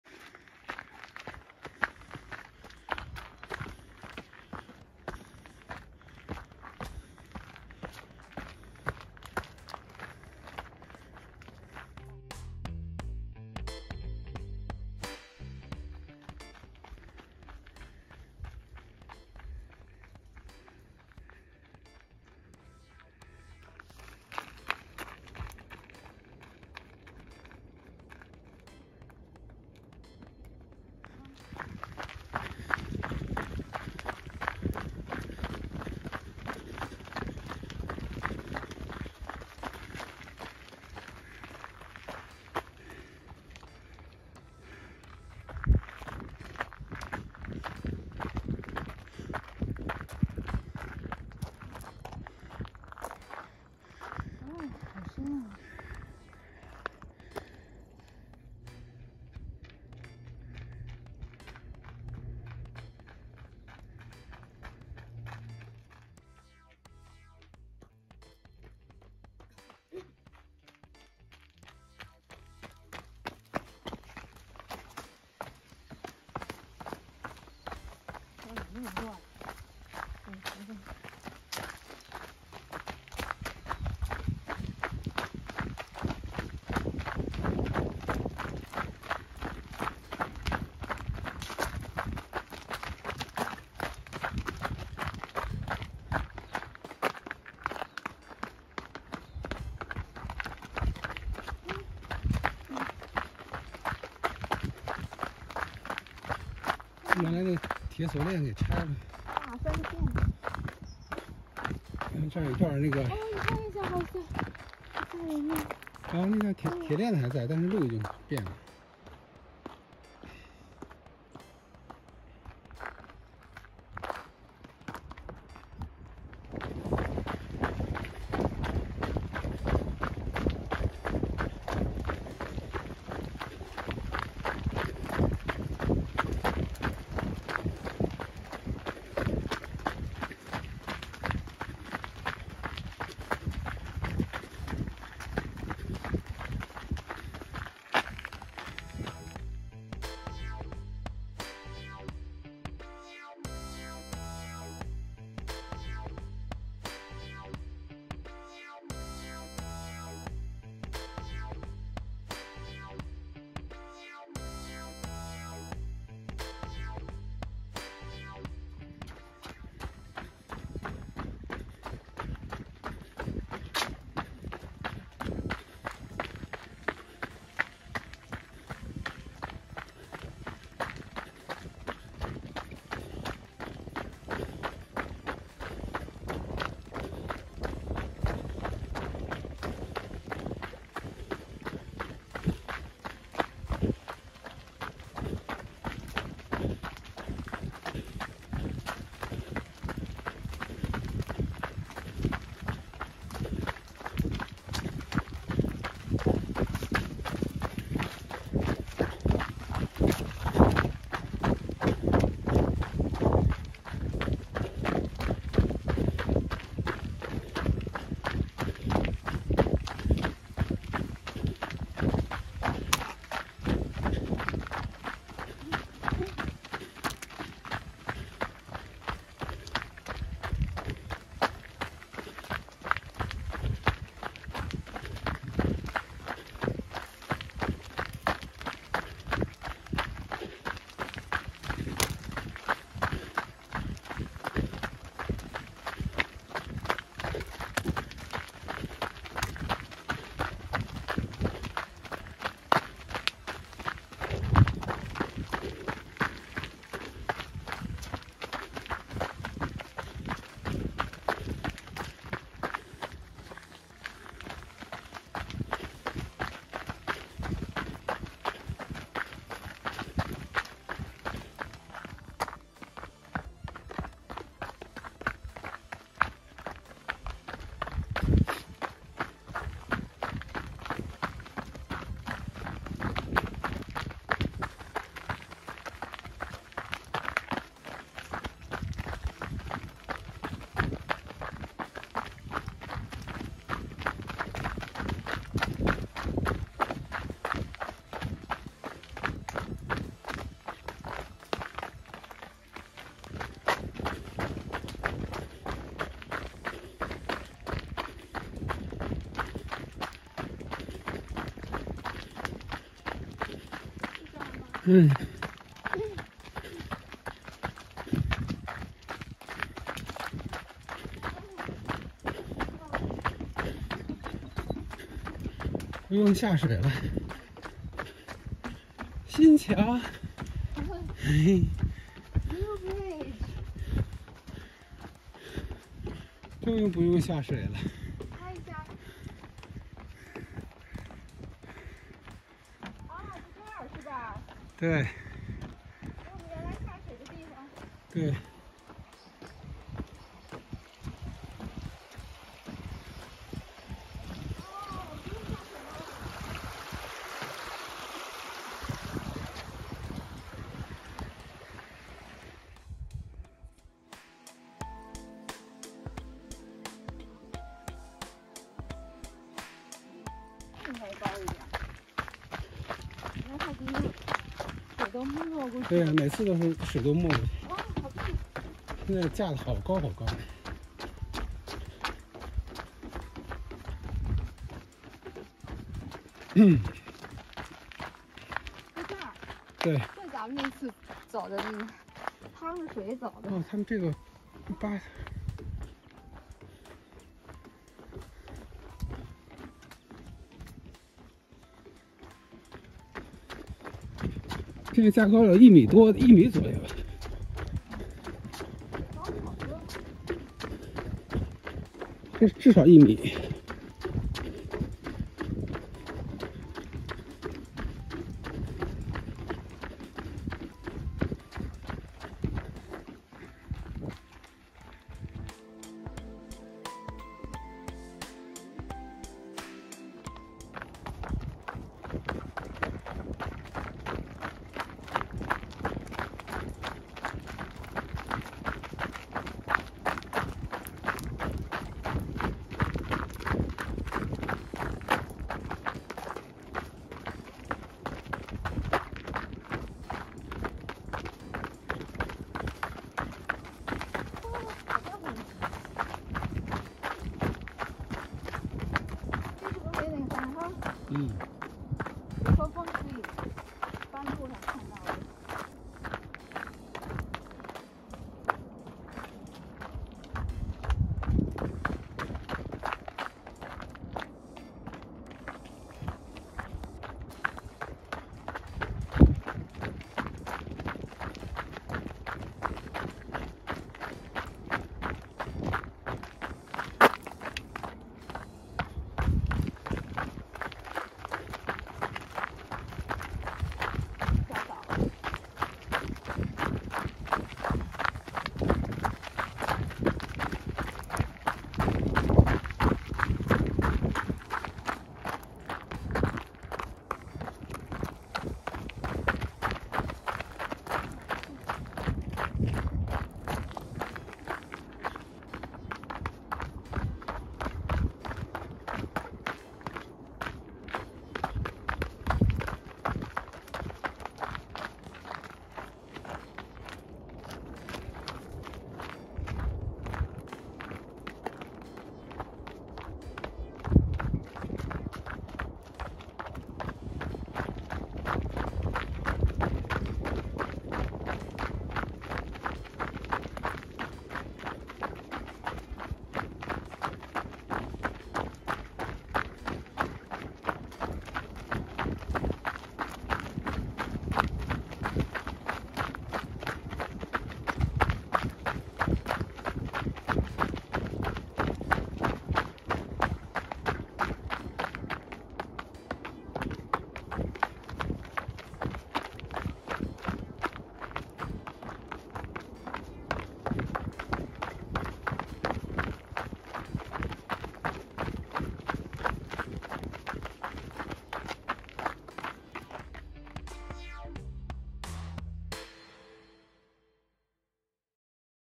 Easy trail running 轻松越野跑 from Whatipu to Pararaha Valley
Morning easy trail running from Whatipu to Pararaha Valley and over back by Gibbons Track, Muir track and Pararaha Vallet Track, in Auckland. 8C°

轻松愉快的越野跑 早上8C°